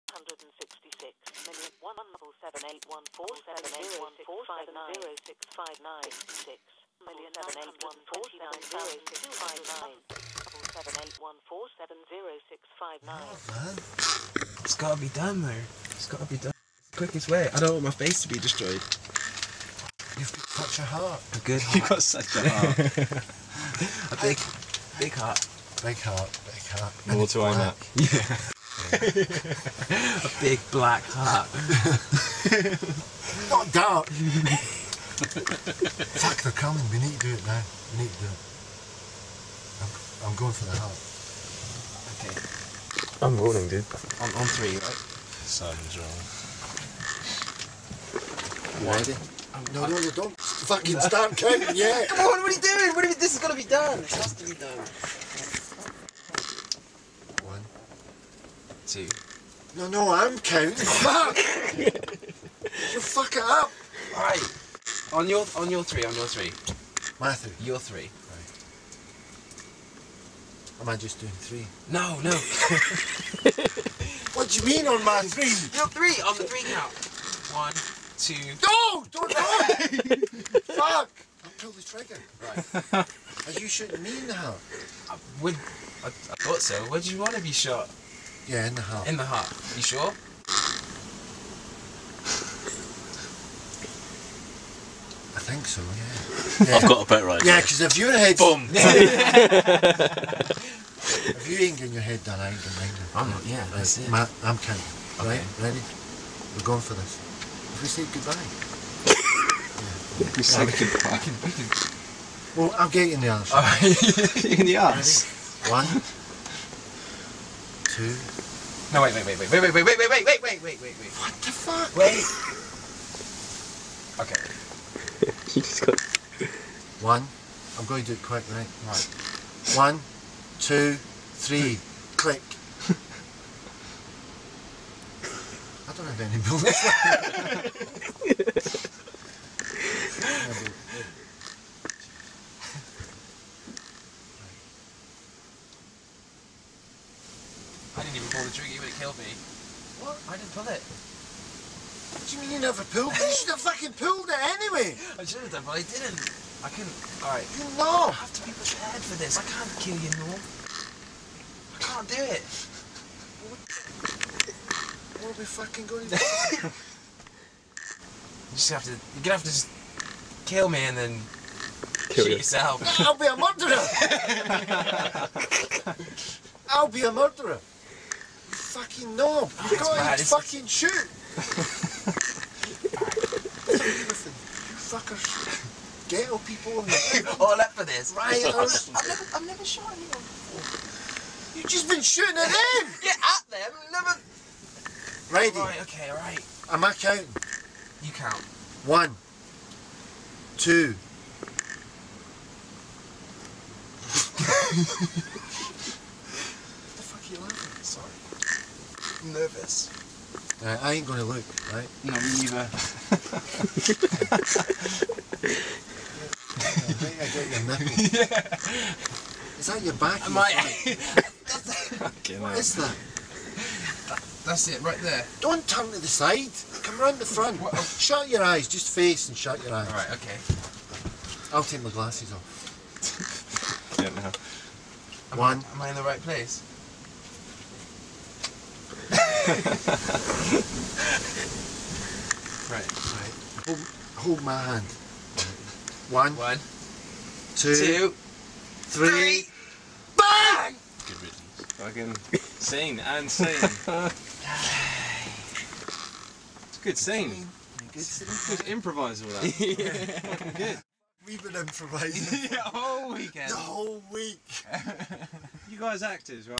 Improvised Scene isNorm & Nathan
Area 51 actors video is about an Improv scene with Norm & Nathan while filming on location in Kent England after a conversation about the film Butch Cassidy and The Sundance Kid when they have to jump off the cliff into the ragging river to escape the Mexican army. Its do or die anyway,what would bring you to the point of killing your best friend ? certain death ? a cruel ending...we never know.